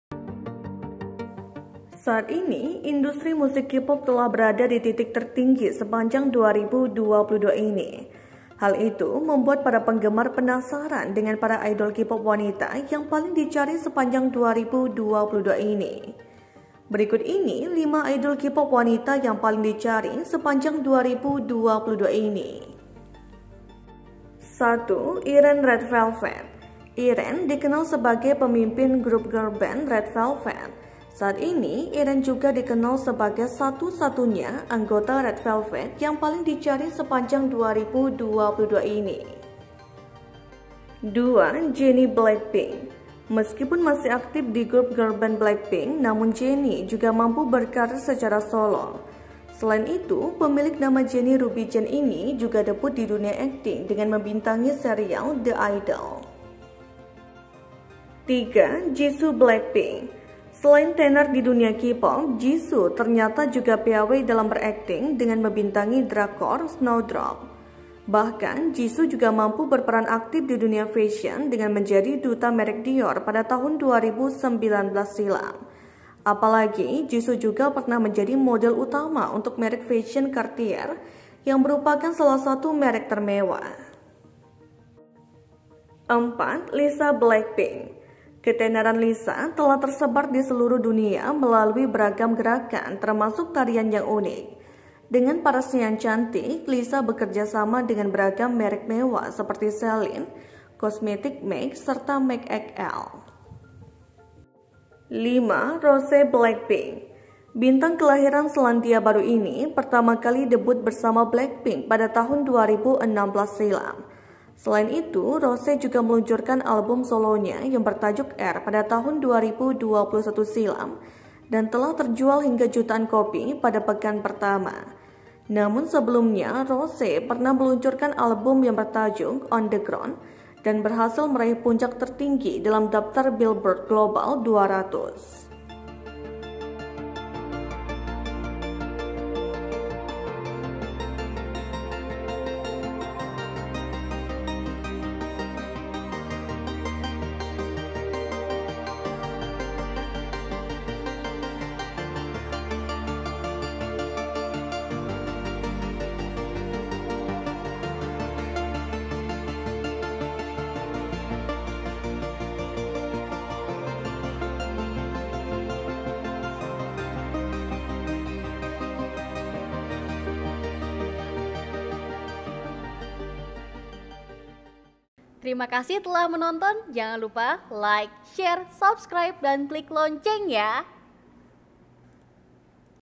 🔴 5 Idol KPop Wanita yang Paling Dicari Sepanjang 2022, Member Blackpink Mendominasi
Saat ini, industri musik KPop telah berada di titik tertinggi sepanjang 2022 ini.

Hal itu membuat para penggemar penasaran dengan para idol KPop wanita yang paling dicari sepanjang 2022 ini.

Berikut lima idol KPop wanita yang paling dicari sepanjang 2022 ini.